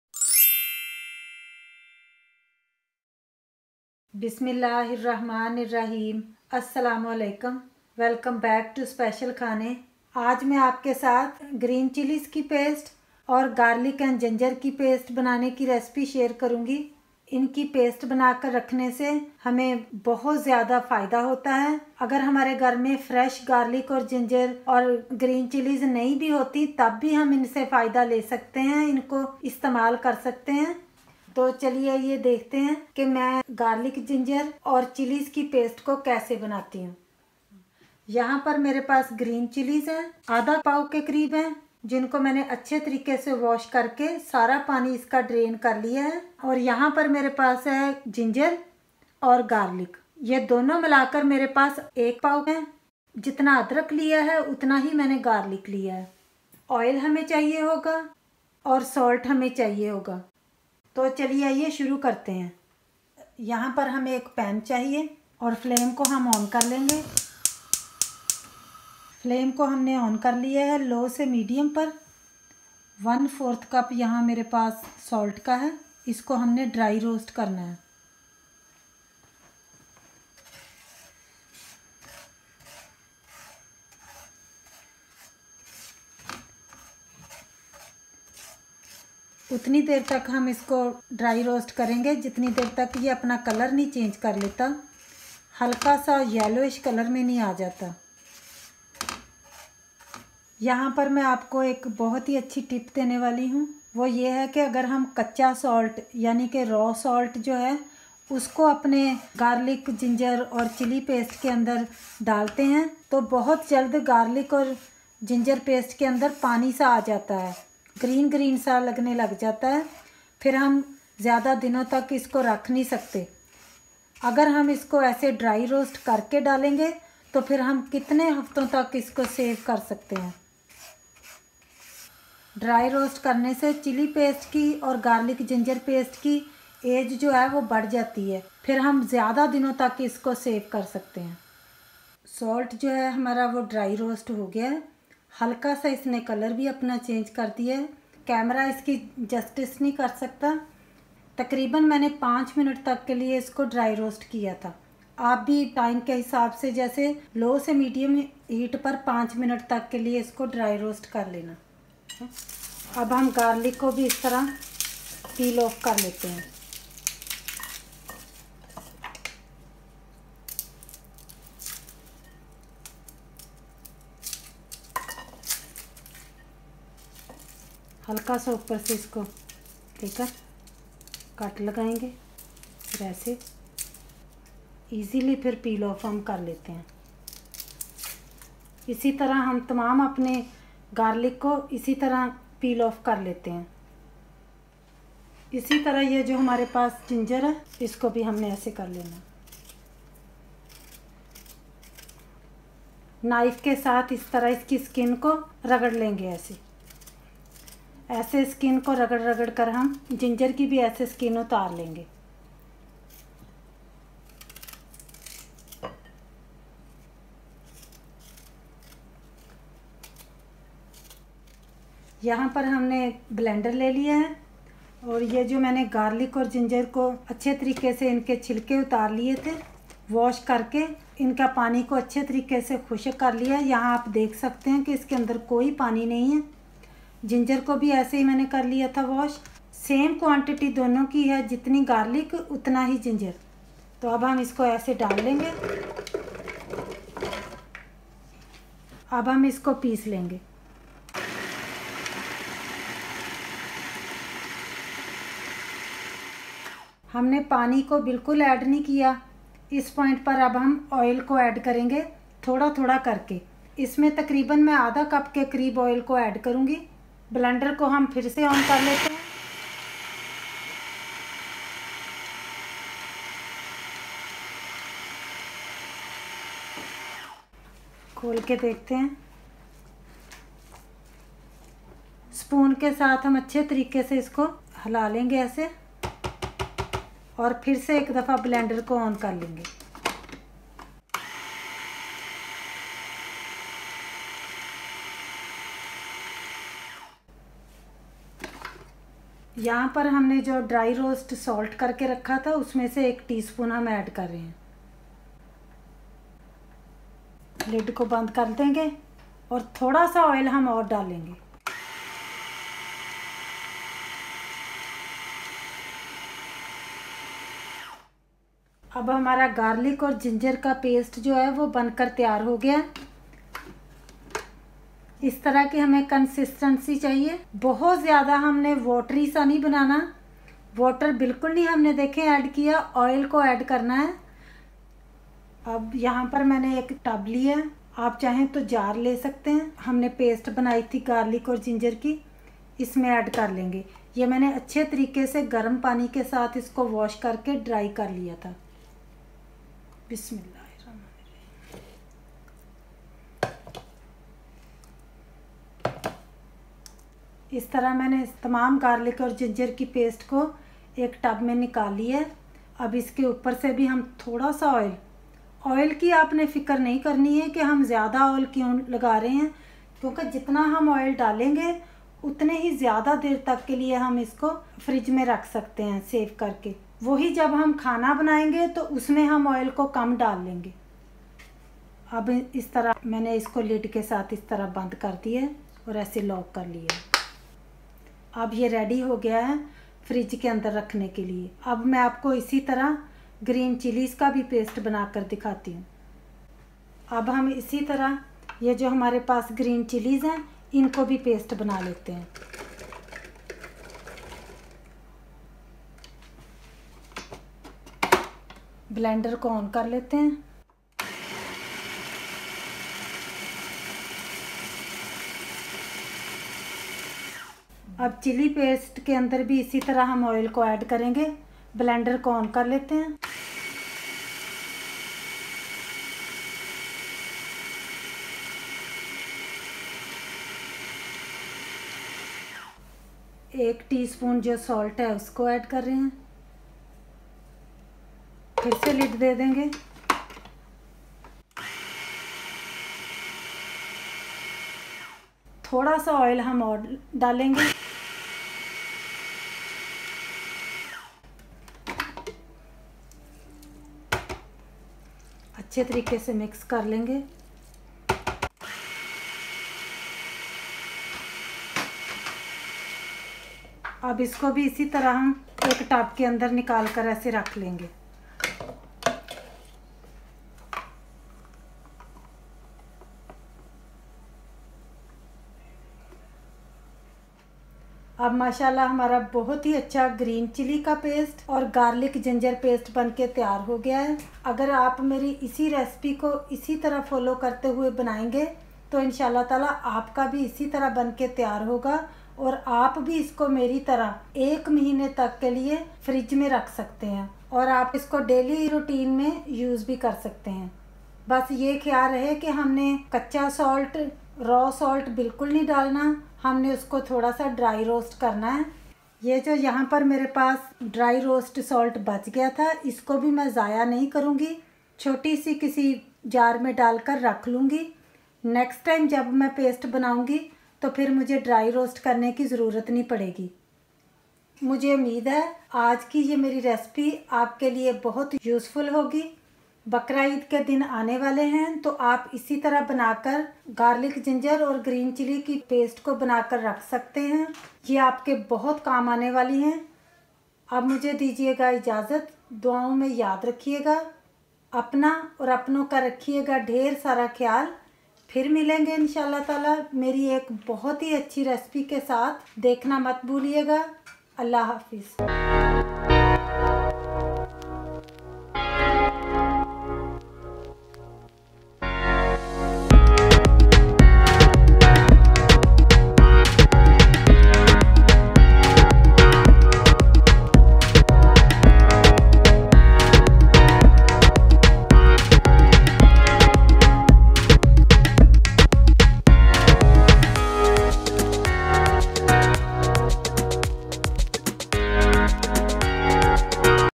How to Make and Preserve Garlic, Ginger and Chilli Paste + Very Unique Tips (EID SPECIAL) *In Urdu*
Ye hai mere Garlic/ginger and Chilli Paste ki Recipe. Umeed hai aapko pasand ayegi!

Today I am sharing my unique tips on how to make and store Garlic/Ginger and chilli paste. it is very helpful to make and store these pastes as they are handy and easy to use.  All the ingredients used are listed in the video in English if needed.

Thank you for viewing my video, I hope you enjoyed it. Please let me know if you liked it in the comments, it will be very much appreciated. __________________________________________________________________________________________